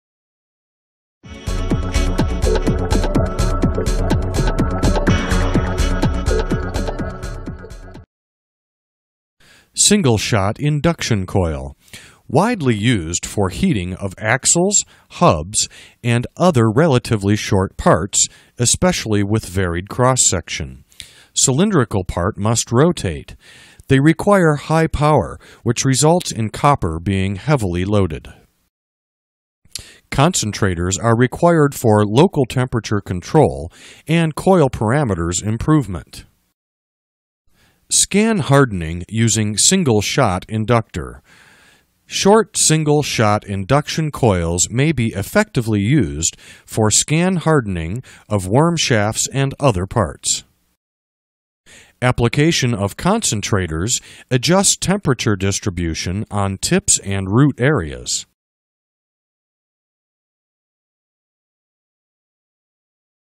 Single Shot Induction Coils
Fluxtrol's Training Course Chapter 3 discusses Single Shot Induction Coils.  Note: Full high resolution video is located